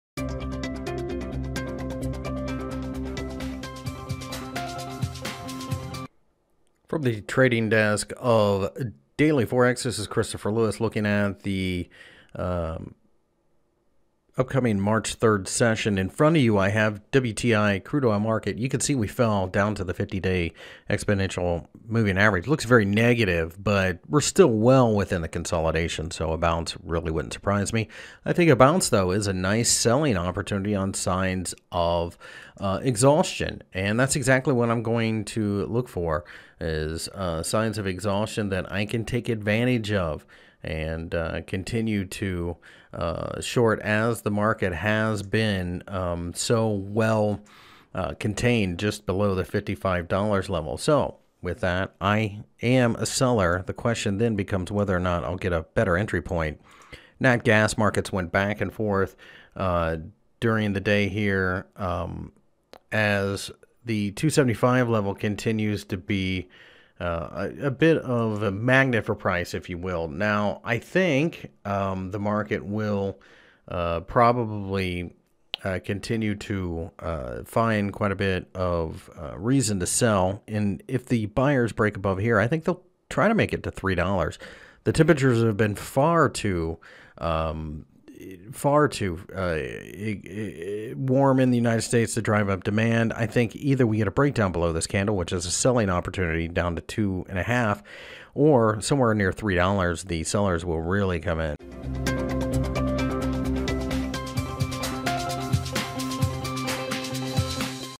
WTI Crude Oil and Natural Gas Forecast March 3, 2017
Oil markets fell significantly during the Thursday session, reaching down to the 50-day exponential moving average. The market has been struggling with the OPEC production cuts and the oversupply happening at the same time. It looks likely that the sellers are going to continue to press the issue, but we would need to see a continuation of the negativity to start selling again. Were essentially in the middle of the consolidation area, so it’s unlikely that it’s going to be an easy move lower. Rallies at this point should be sold off though, so I would be looking for short-term rallies that show signs of exhaustion in order to get short of this market yet again.